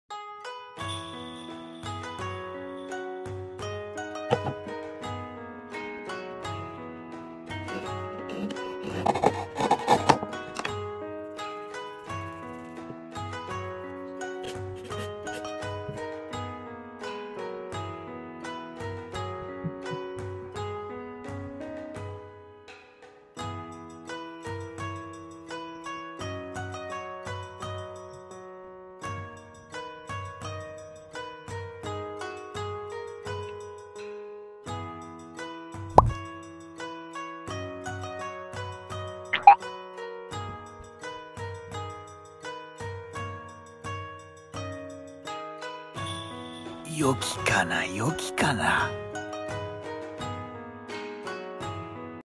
【OKINA MAKING】Make a traditional Japanese mask "OKINA"
How to make a wooden mask taught by Japanese mask artist Hiromitsu